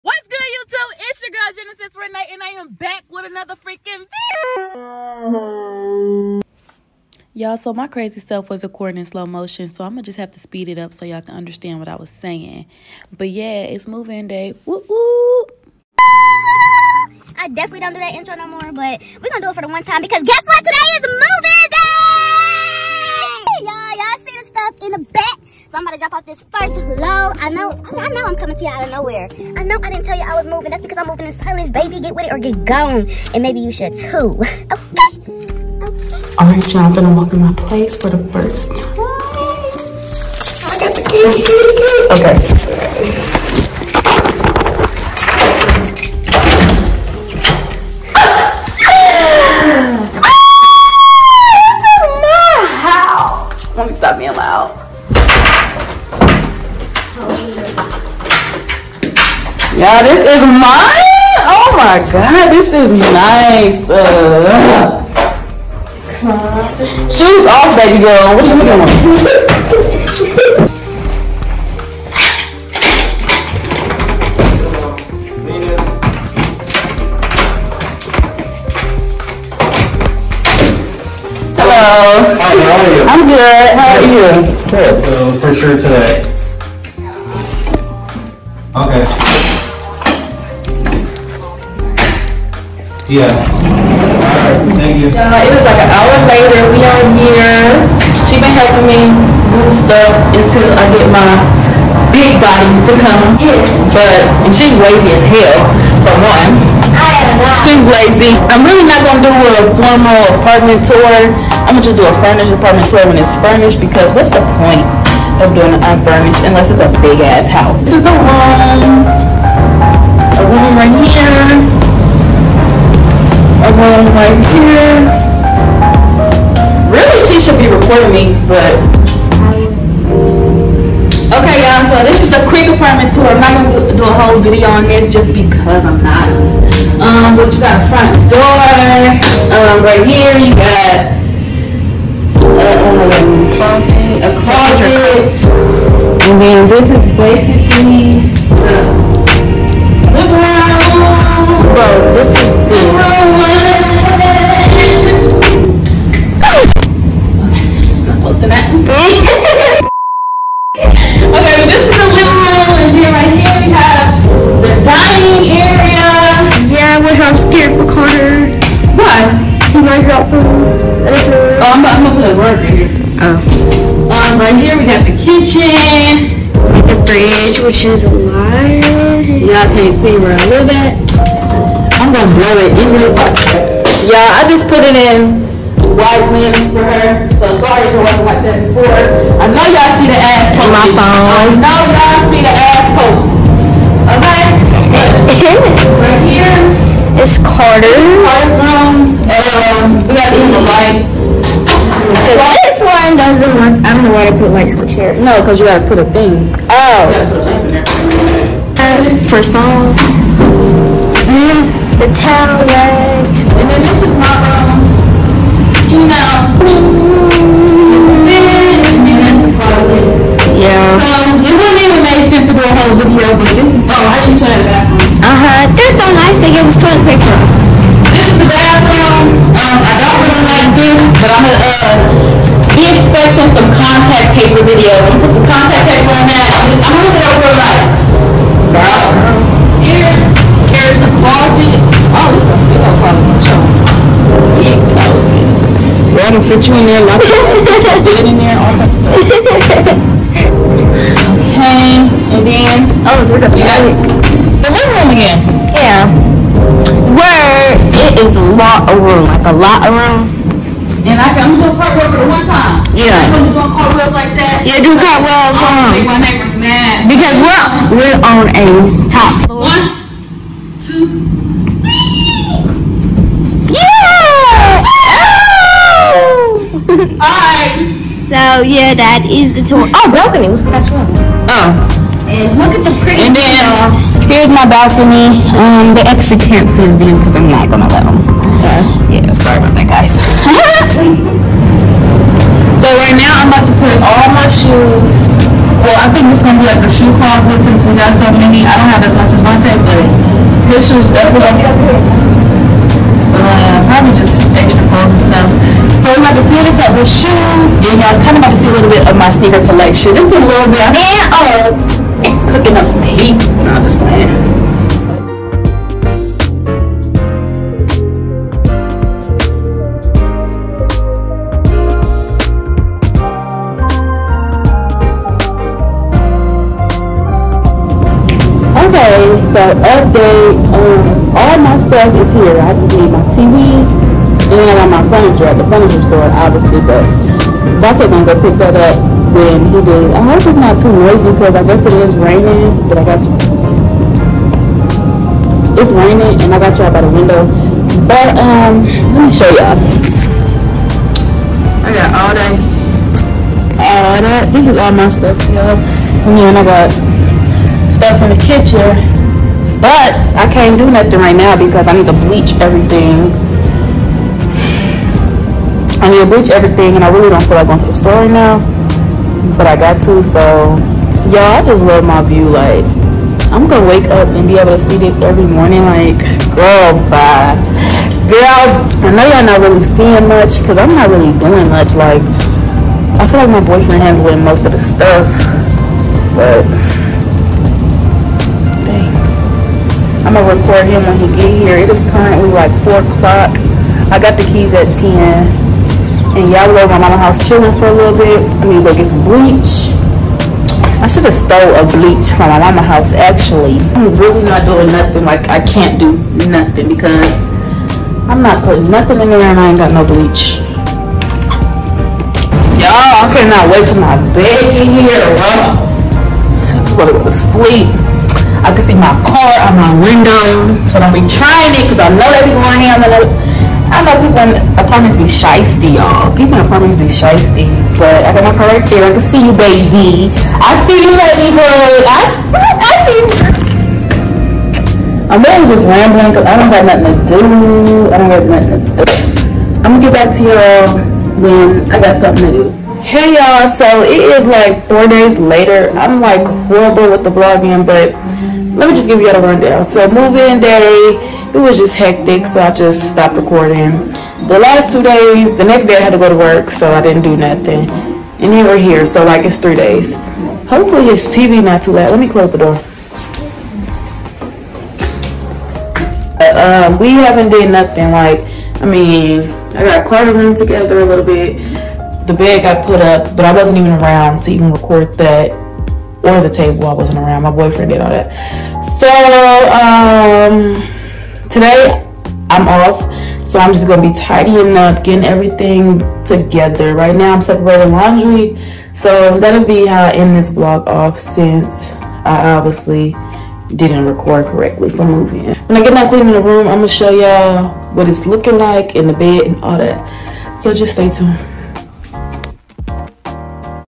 NEW APARTMENT! Move In Day + Mini Unfinished Apartment TOUR 2021
WATCH VIDEO IN 1080p

Karter’s Instagram: @Iamkingkarter